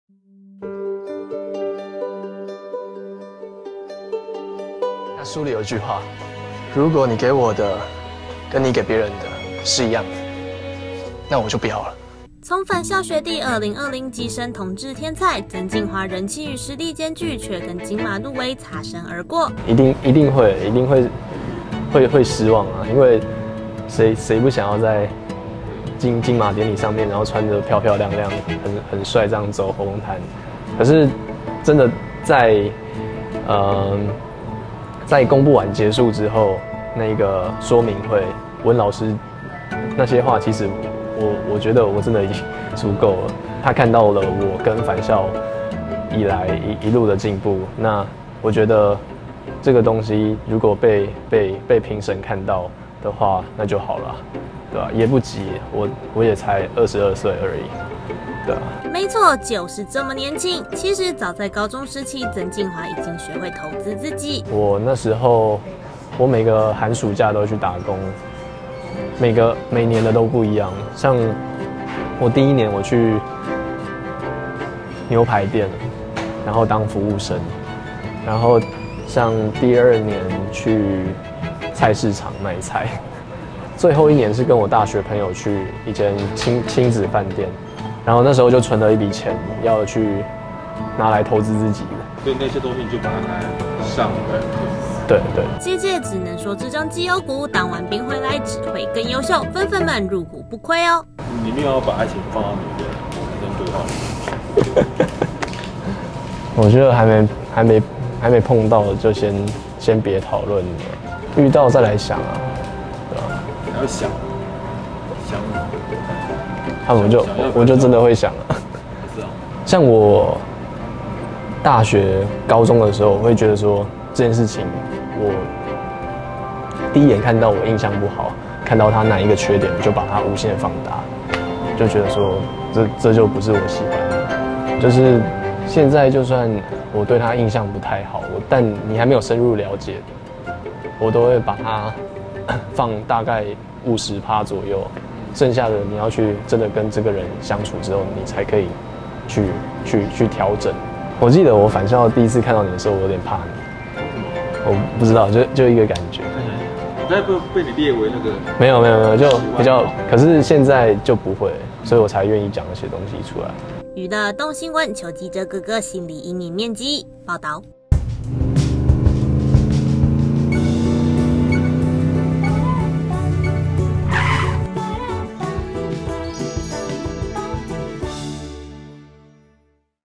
22歲曾敬驊助家賣早餐批發糕點 《刻在你心底的名字》後等當4個月大頭兵 專訪 | 蘋果新聞網
22歲曾敬驊去年端出首部大銀幕處女作《返校》，不但擔綱男主角且狂吸2.6億票房；今年第2部電影《刻在你心底的名字》上映14天已賣5600萬票房，又掀高熱度，有望衝上年度冠軍，他本身的人氣已再翻倍。他接受《蘋果新聞網》專訪，坦言自己很幸運。2趟電影旅程像一串華麗冒險；但離開鎂光燈，他依然是那個宜蘭長大、跟家人講台語、家裡開早餐店兼糕點批發的純樸大男孩，所以日前中秋節正值電影宣傳，他仍抽空告假回家幫忙包裝糕點。近期努力宣傳《刻在》同時，也正乖乖等兵單，他已抽過籤，坐等4個月的陸軍洗禮。

曾敬驊的崛起當然有很多外在的幸運成分，但有一部分是他發自內在的偏愛選擇。高三時，他是游泳校隊一員，面臨大學抉擇時，他思考到游泳並不能長久發展，於是選擇電影系。他說人生看的第1部電影是《鐵達尼號》，但選擇志願前再勾起他對電影樂趣的片是《大尾鱸鰻》，他說這齣喜劇讓他又覺得電影很好玩，於是考進高雄的義守大學電影系，選擇編導組；到了後期，他也對表演有了朦朧遠景，開始趁休假上台北自費參加1對1表演課，因為他說校內的表演課是大班制，幫助有限。